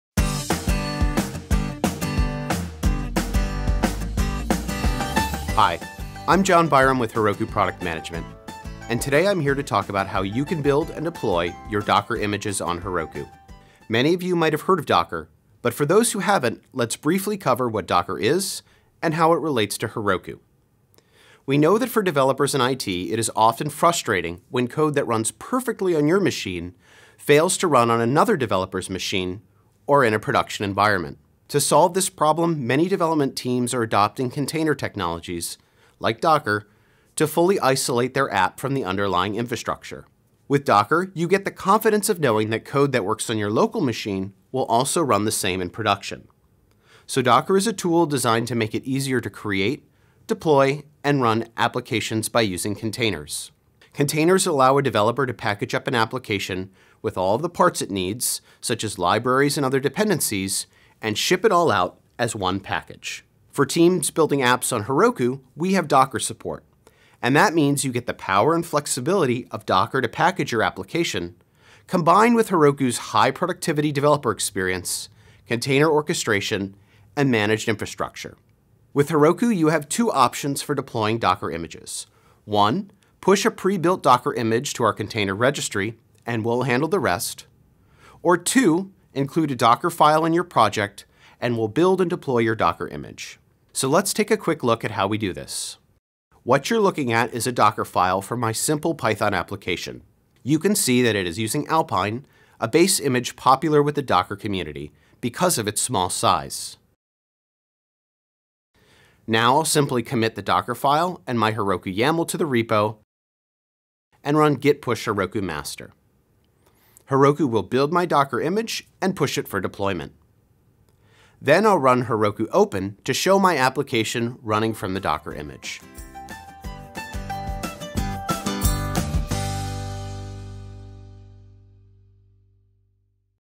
Heroku Basics: Docker
Many development teams adopt container technologies like Docker to fully isolate their apps from the underlying infrastructure. In this video we learn how developers can deploy Docker images on Heroku.